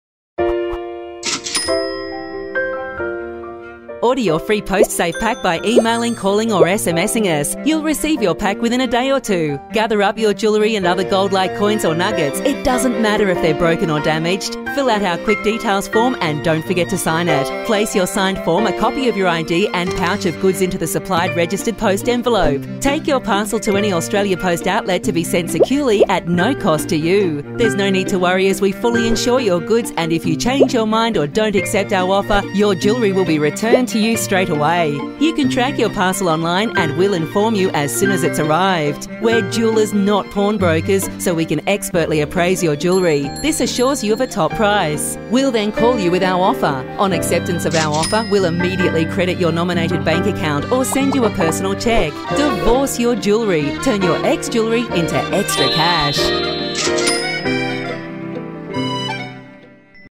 Divorce Your Jewelry Postal Version
Second Animation for DYJ, turn your ex-jewelry intro ex-tra cash. Postal version